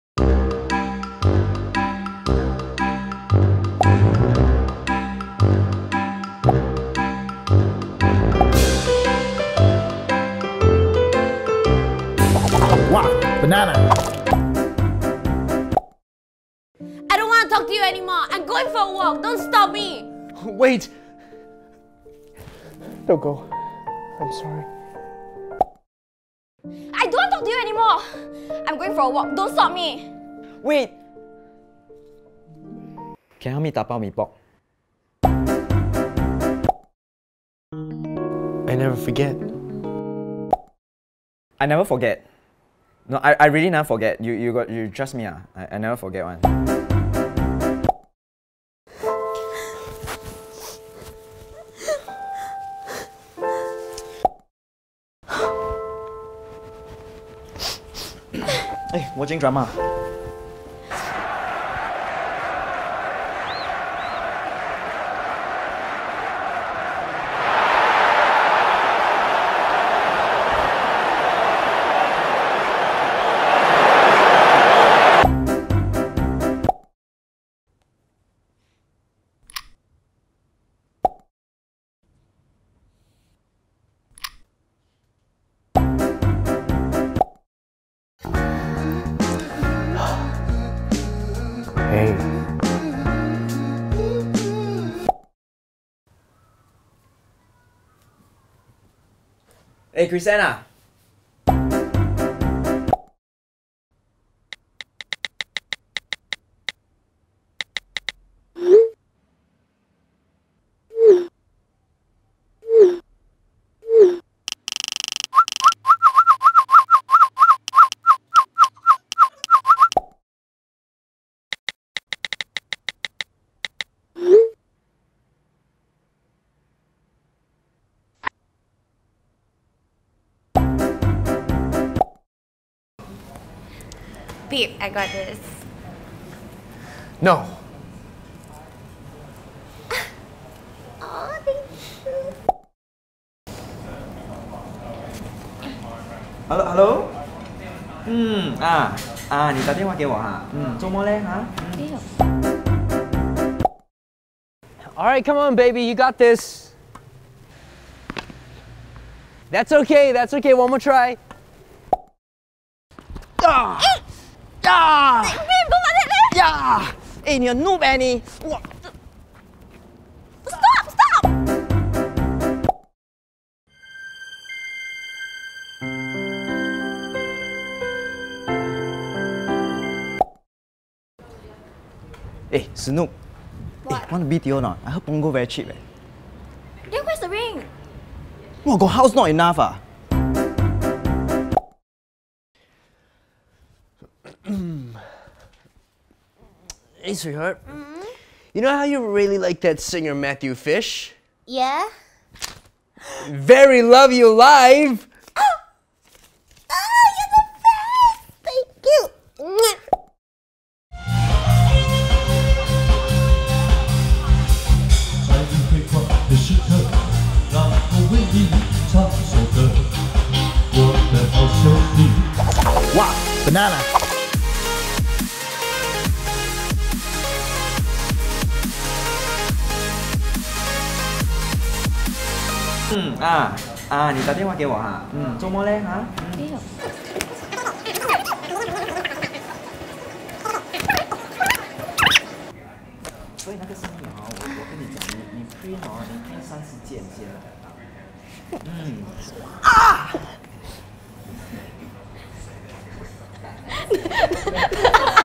Her Boyfriend vs Your Boyfriend
There are good boyfriends, followed by so-so boyfriends, and then there is your boyfriend.

Produced and Written by Lingyi Xiong @Blingyi
Filmed by Jason Hau @Jasonkokotan
Edited by Bryan James Kwa @bryanjameskwa

Starring:
Eden Ang @Eden_Ang
Benzo @thisisbenzo